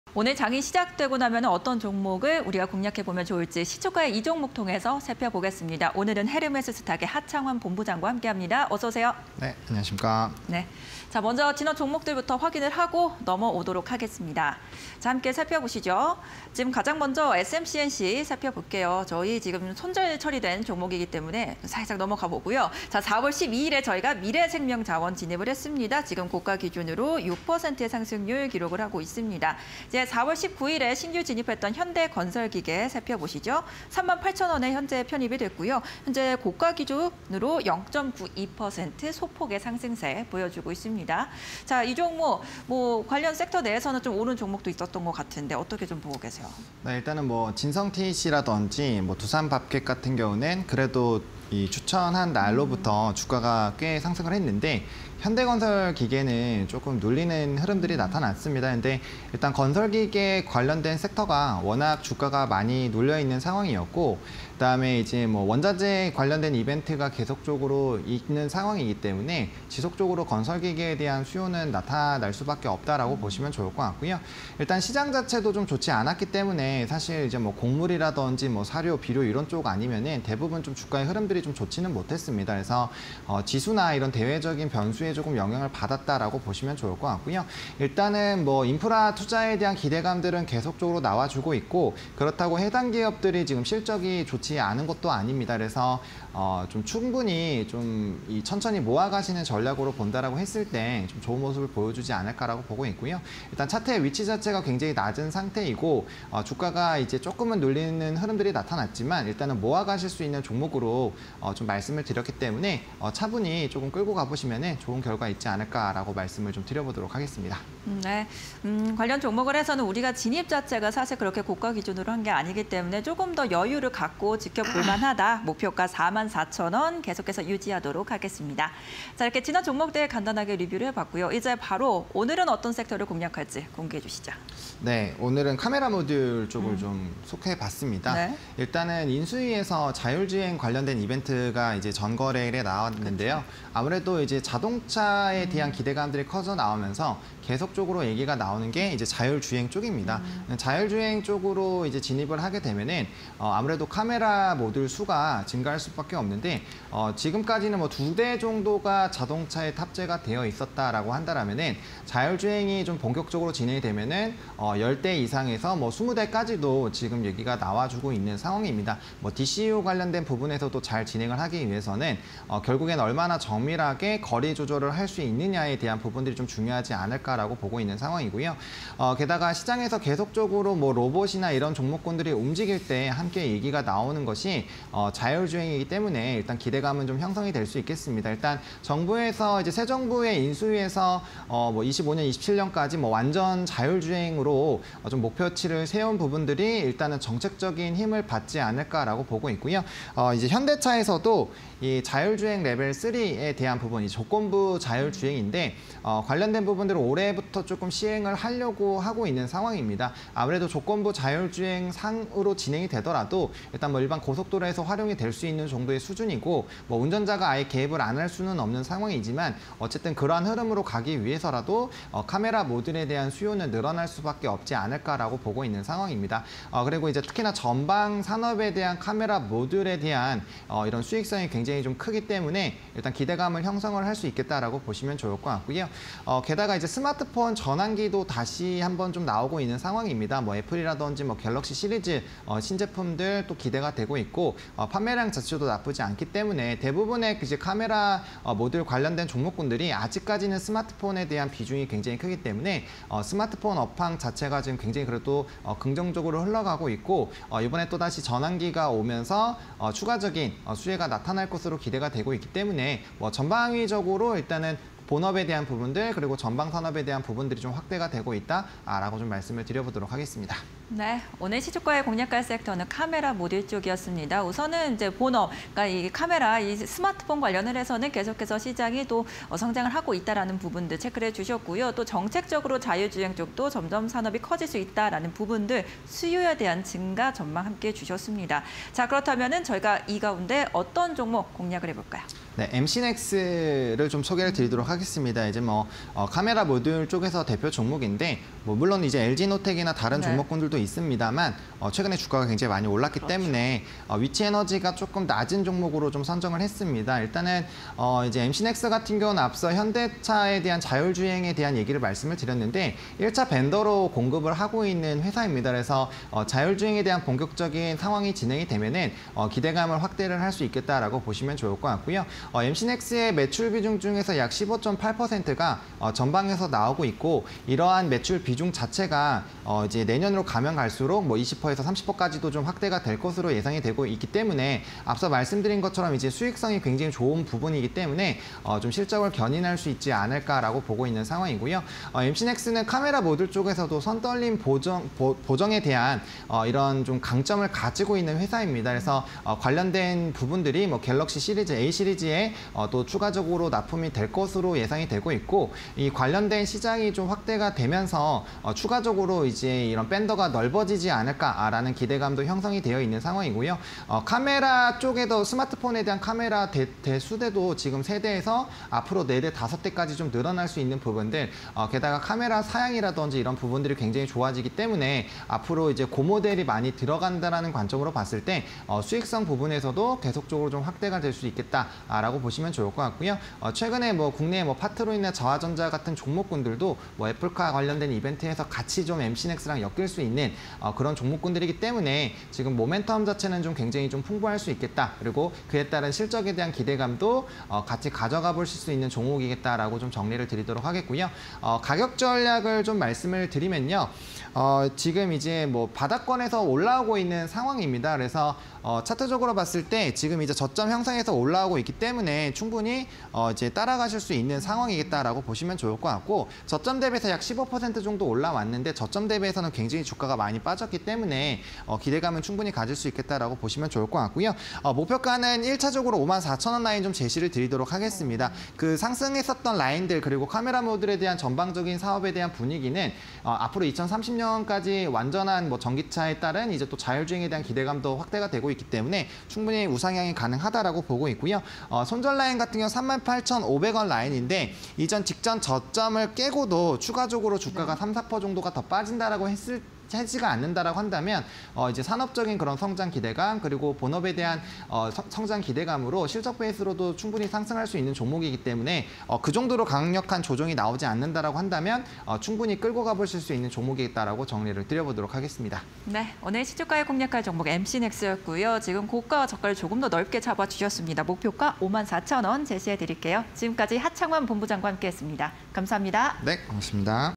엠씨넥스(097520)... 카메라 모듈 관련주_시초가에 이 종목 (20220425)
카메라_모듈 관련주
- 자율주행 등 관련 산업 성장 기대감 확대
- 스마트폰 전환기·신제품 출시
- 고사양 수요 확대로 판매 단가 상승 기대감
- 자율주행·도심항공교통 등 미래 모빌리티 육성
- 현대차, 올해부터 '레벨3' 양산차 보급 예정
- 이동통신3사 모빌리티 분야 선점

엠씨넥스(097520)
- 스마트폰·자율주행 확대로 카메라 모듈 수요 증가
- 전장용 카메라 모듈 매출 확대 기대
- 현대차, 2022년부터 레벨3 양산차 보급 예정
- 카메라·라이다 등 인식 대상 및 정확도 향상 목표
- 전사업 고성장 예상… 올해 실적 기대감 확대
- 올해 삼성전자 스마트폰 판매 증가 및 손떨림 보정 관련 매출 증가 기대
- 자율주행 관련 전장향 카메라 매출 증가
- 갤럭시A 시리즈 등 영역 점유율 증가 기대
- '손떨림 보정' 관련 경쟁력 확인
- 하반기 제네시스 등 전기차 카메라 등 매출 기대